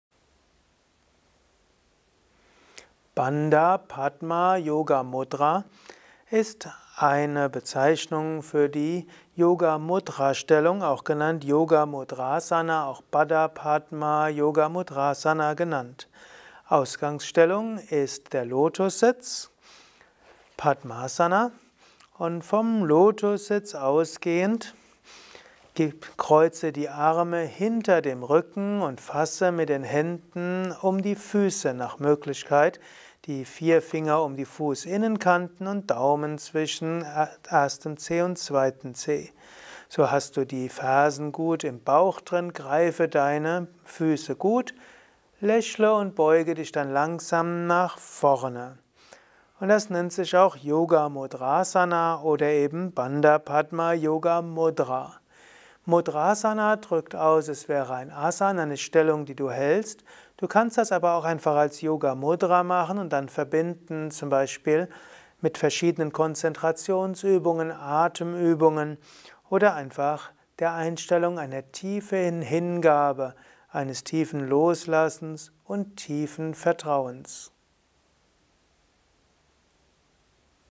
Bandha Padma Yogamudra - Asana Lexikon 282
Bandha Padma Yogamudra bzw. बन्धपद्मयोगमुद्रा, bandha-padma-yoga-mudrā: Erläuterung, Vorführung und Übungsanleitung. Bandha Padma Yogamudra gehört im Yoga zu den Vorwärtsbeugen, Asanas im Sitzen.

Bandha Padma Yogamudra ist eine Variation von Padmasana